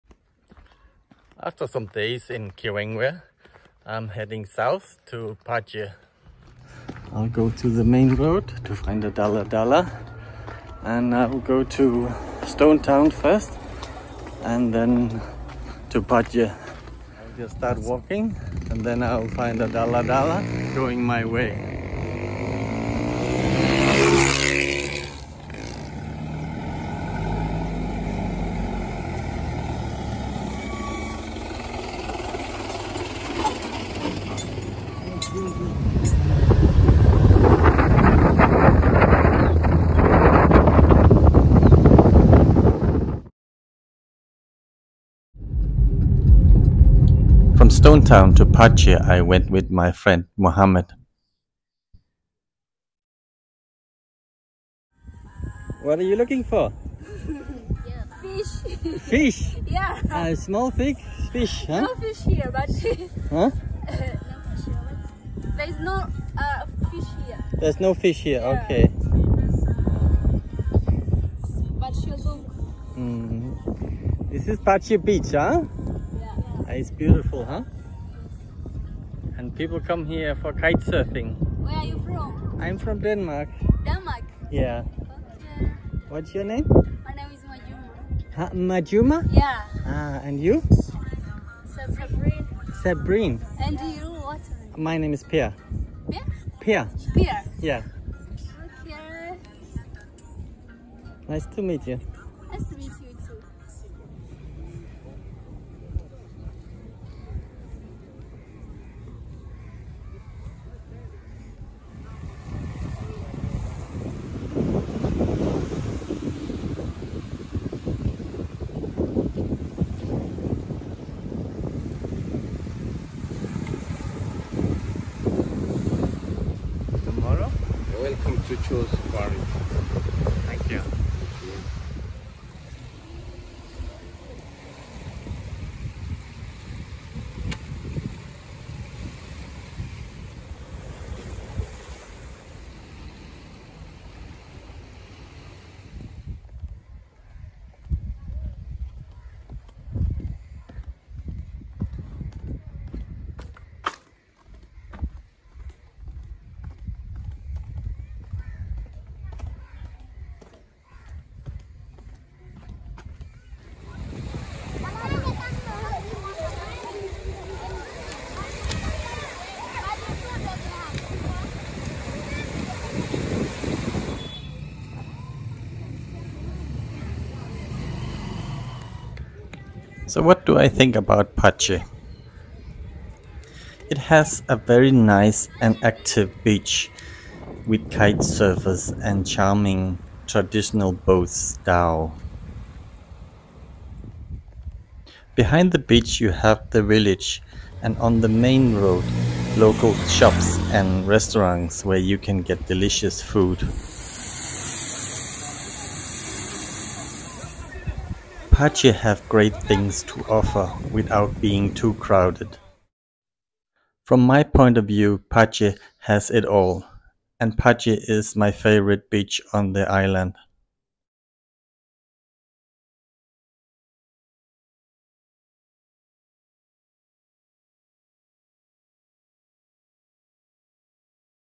BEACHES OF ZANZIBAR - Part 3 - Paje
I want to go around Zanzibar to visit its famous and amazing beaches. I will use local transport to feel "Africa".

The beach area with its hotels are made to give the tourists a good experience during their vacation.
But will there be the sense of "Africa" ​​in the area?

My third stop is Paje on the east cost.

Paje is a village on the Tanzanian island of Unguja, part of Zanzibar. It is located on the southeast coast between the villages of Bwejuu and Jambiani..

See my opinion on Paje at the end of this video.